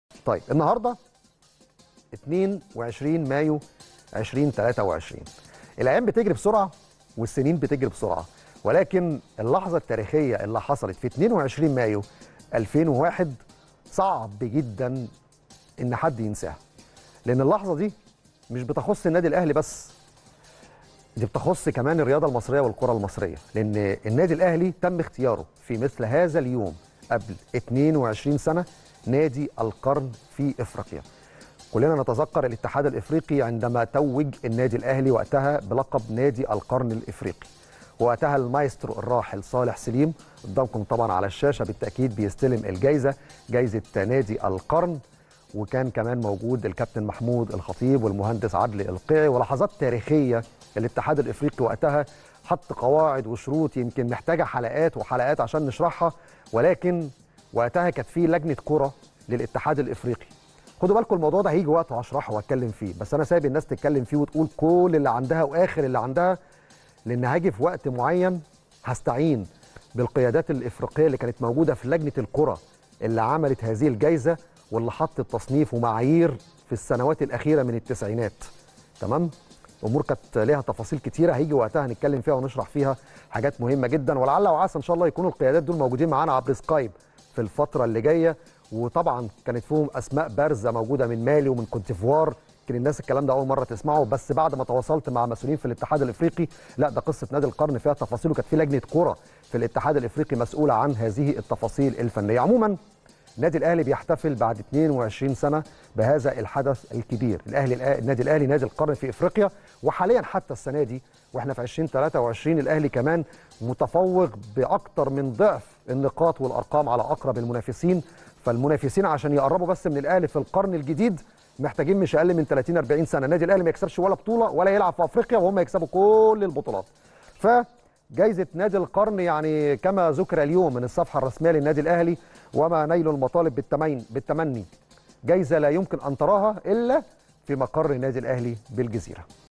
في مثل هذا اليوم عام 2001 🗓️ الأهلي يتسلم جائزة نادي القرن في افريقيا 🌍🏆 في حفل ضخم بمدينة جوهانسبرج
في مثل هذا اليوم عام 2001 🗓️ الأهلي يتسلم جائزة نادي_القرن في افريقيا 🌍🏆 في حفل ضخم بمدينة "جوهانسبرج" 🦅 | التريند